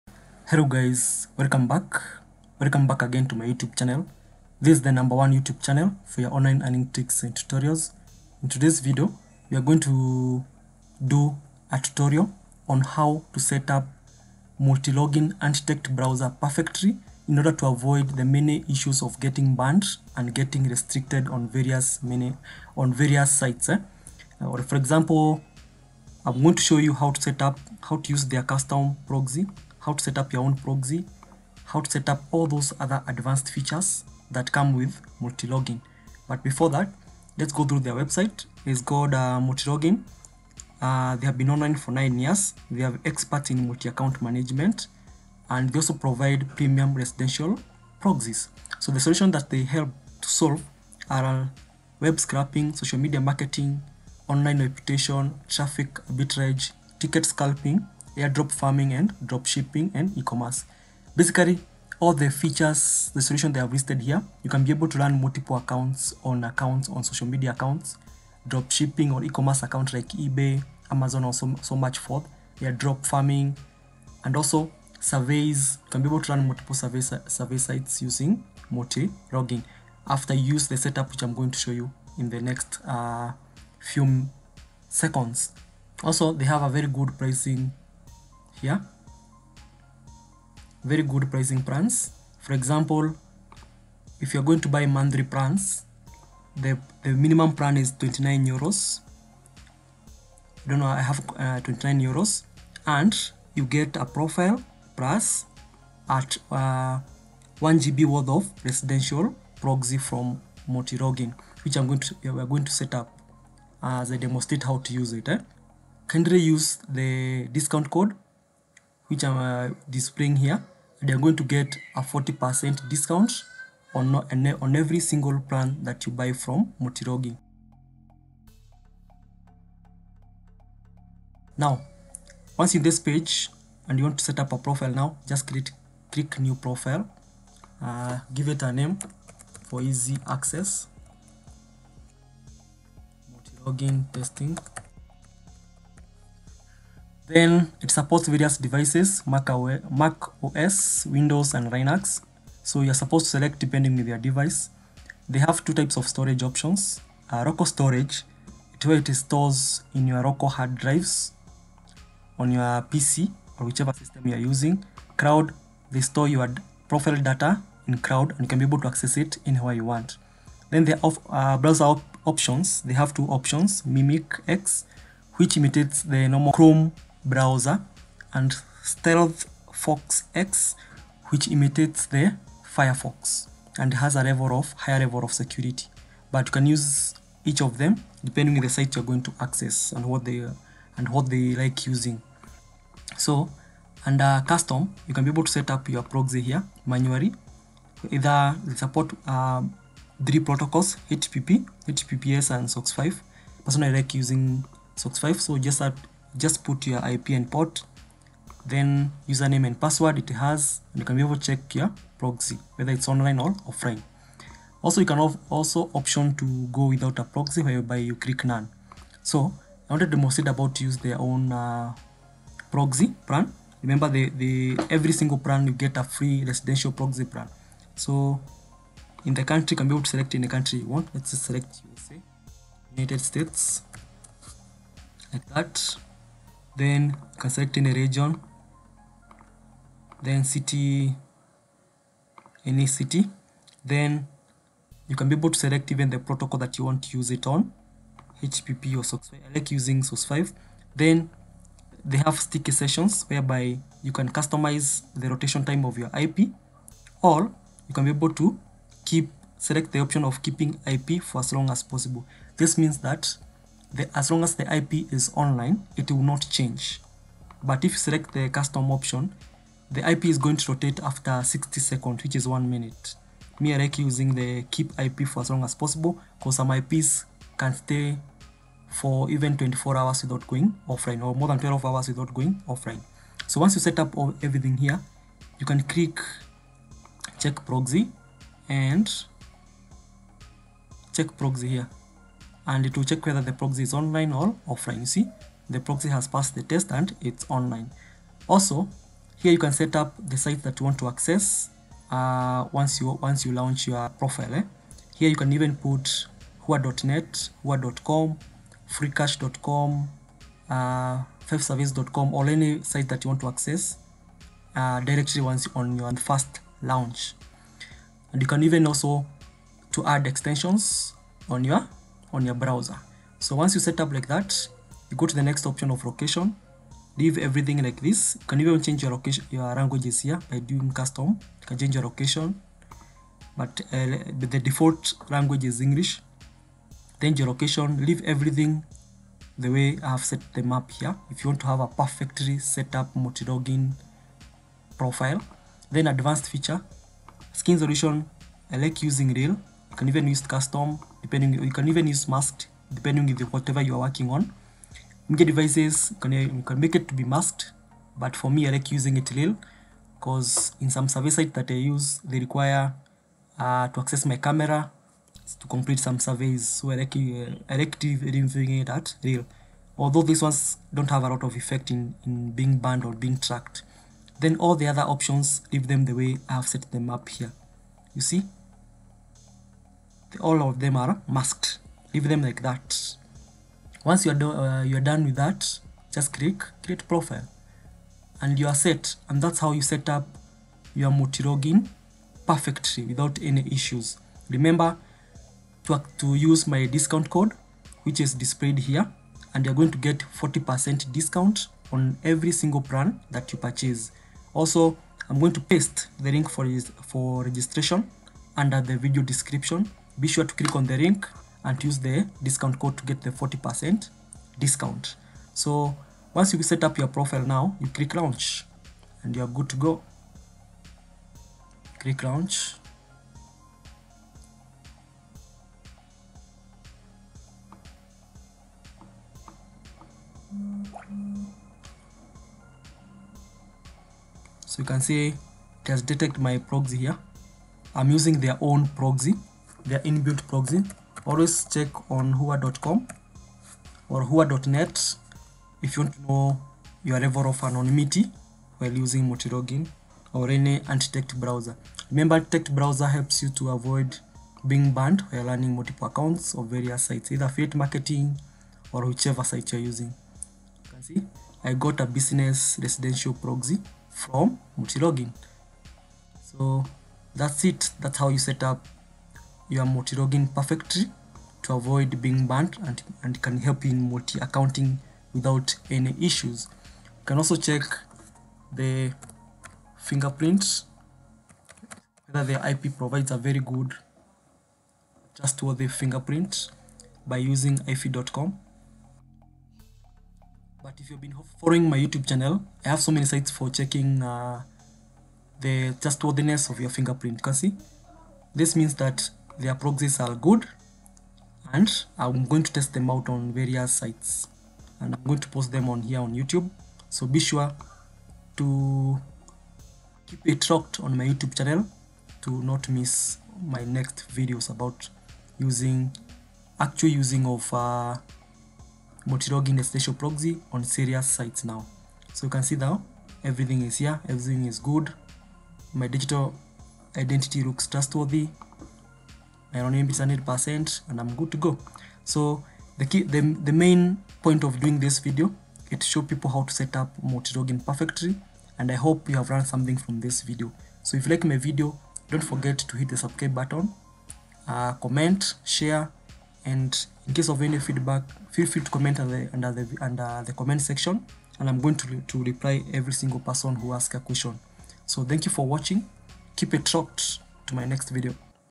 Multilogin Antidetect Browser EXPOSED Top Residential Proxy Secrets
Description:
In this video, we'll show you the step-by-step process of setting up MultiLogin, a powerful tool that allows you to manage multiple accounts from a single dashboard. With MultiLogin, you can easily switch between accounts, manage your passwords, and stay organized. So, what are you waiting for? Watch this video and become a MultiLogin expert today!

ABC s5 Proxy: Affordable & Reliable Socks5 Proxies Provider|911.re Alternative

JOIN US ON OTHER PLATFORMS:
My telegram username: @izohk